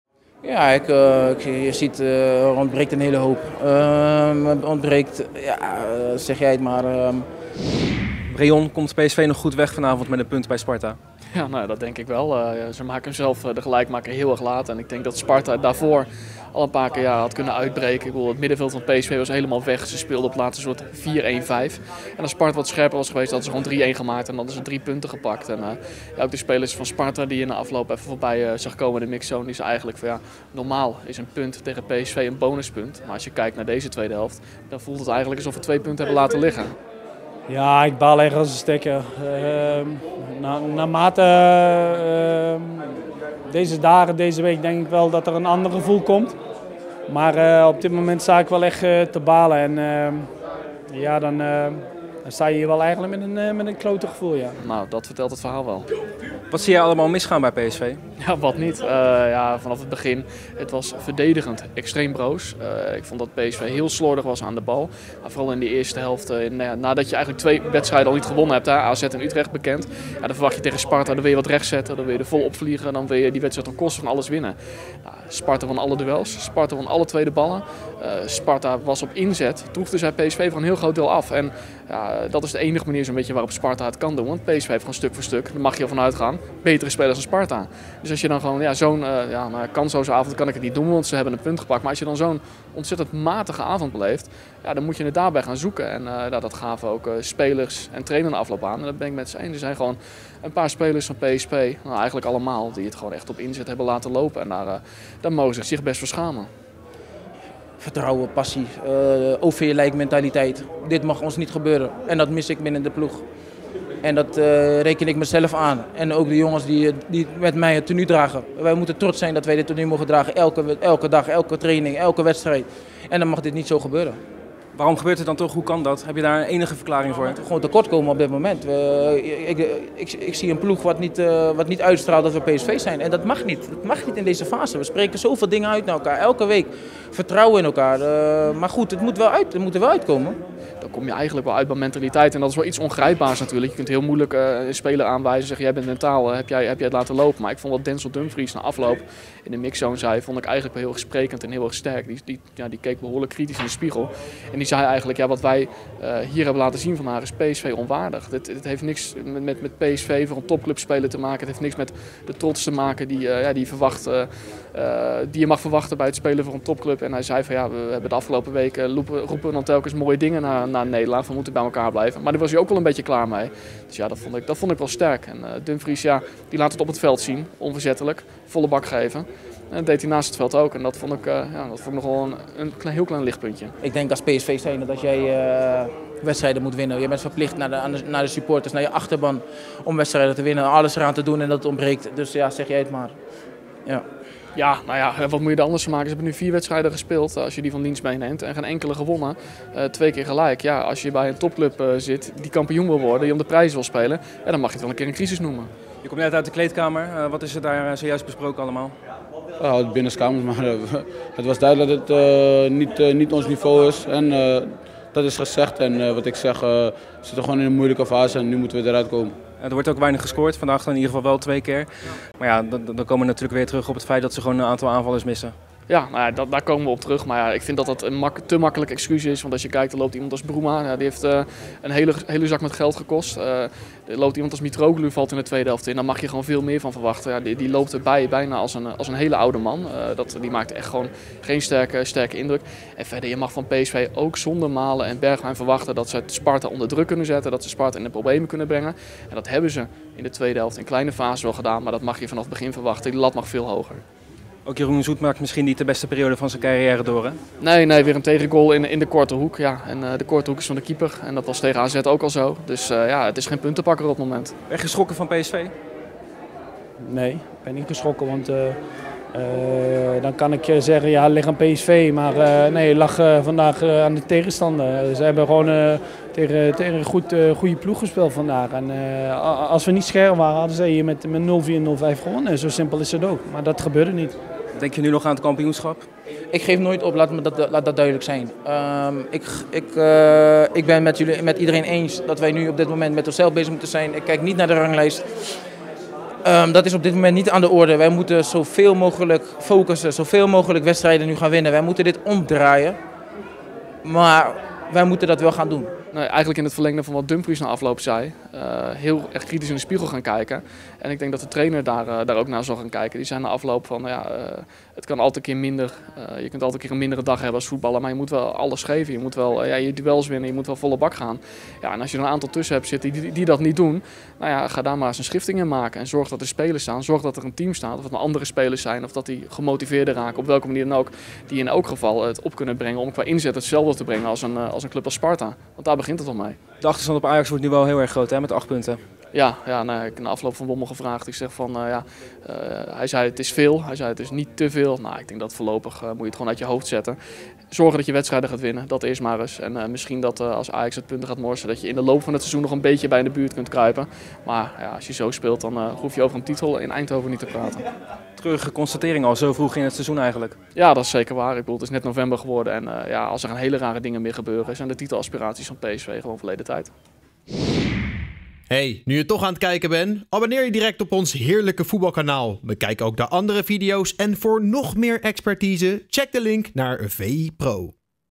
Titelaspiraties In Eindhoven Verleden Tijd: 'PSV Moet Zich Schamen!'
PSV heeft zaterdag wederom dure punten laten liggen in de strijd om het kampioenschap. Uit bij Sparta kwam de ploeg van trainer Mark van Bommel niet verder dan een 2-2 gelijkspel. De achterstand op Ajax bedraagt nu liefst acht punten. VI-verslaggever Reon Boeringa blikt met Matthijs Vegter terug op de avond, evenals Denzel Dumfries, Pablo Rosario en Bryan Smeets.